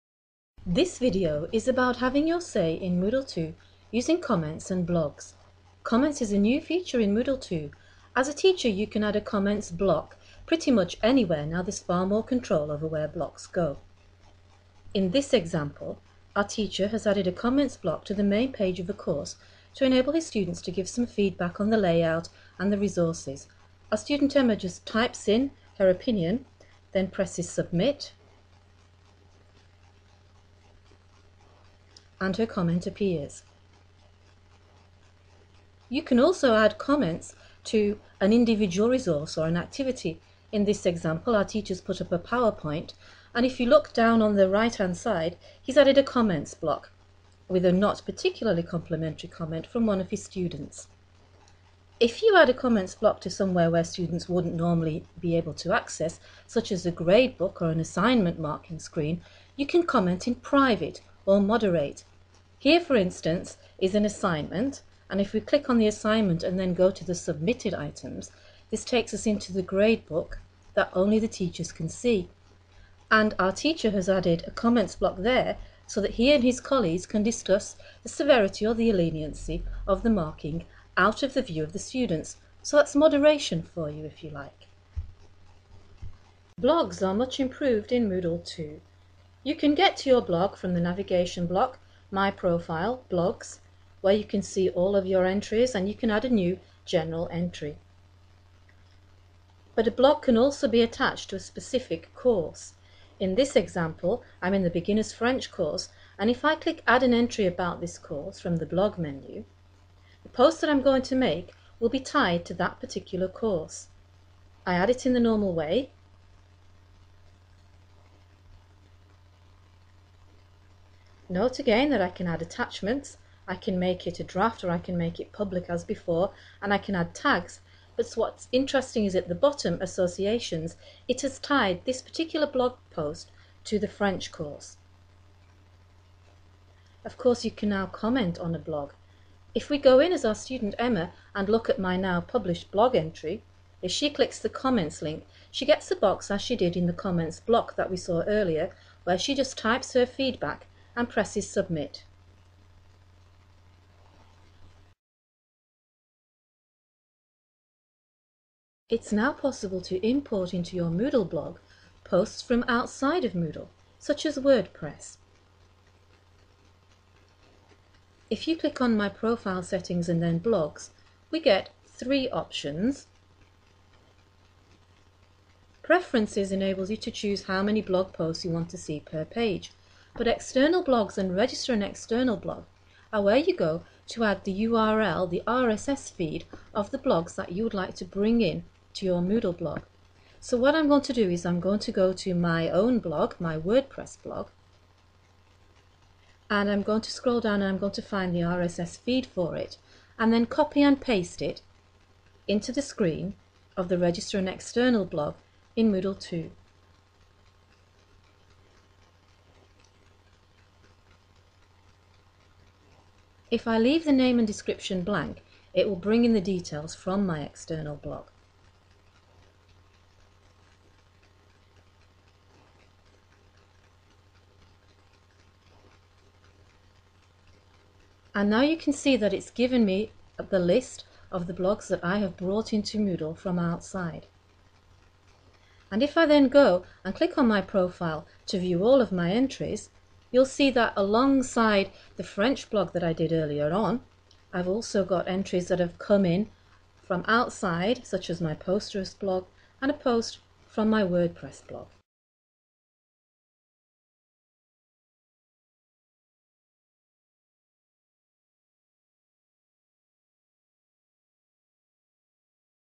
Comments and Blogs in Moodle 2.0
Having your say in Moodle 2.0 using comments and the improved Blog